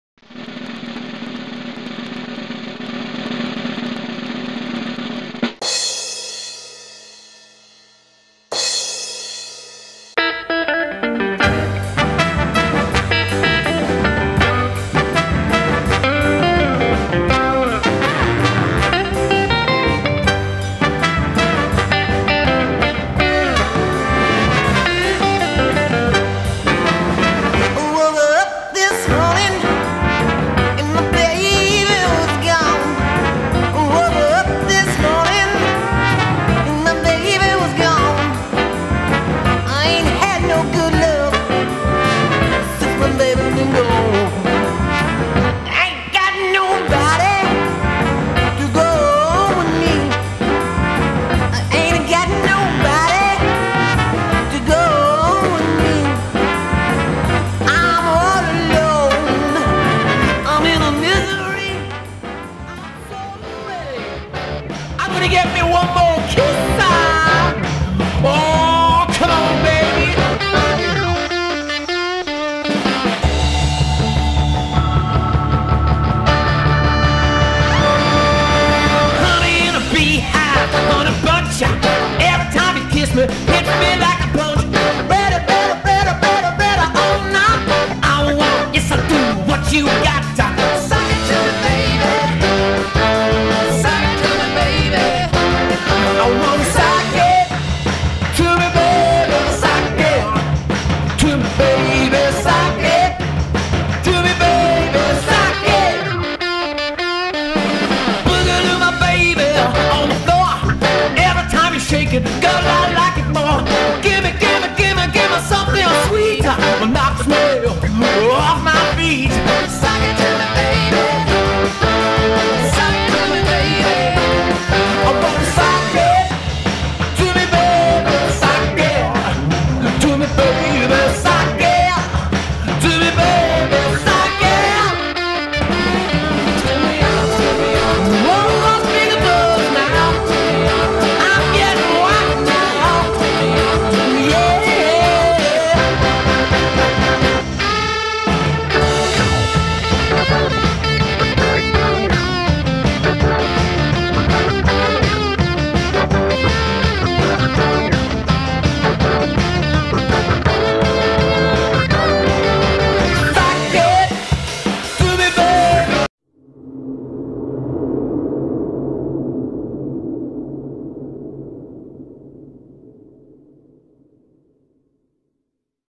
SOL HAUS & THE SPOKESMEN
This eight piece outfit with horns delivers a raucous batch of up-tempo thumping Northern Soul, the most danceable music ever conceived. A bit of rock'n'roll thrown in for good measure to ensure the sets variety and energy is maintained to a very high standard. If you can't dance to this, you don't have feet! We have a great mix of lesser known covers from the likes of Etta James, Ray Charles, Aretha Franklin and much more. These songs are handpicked to ensure the energy never fades, the mix is constantly rising until there isn't a single person who can avoid the lure of the dance floor.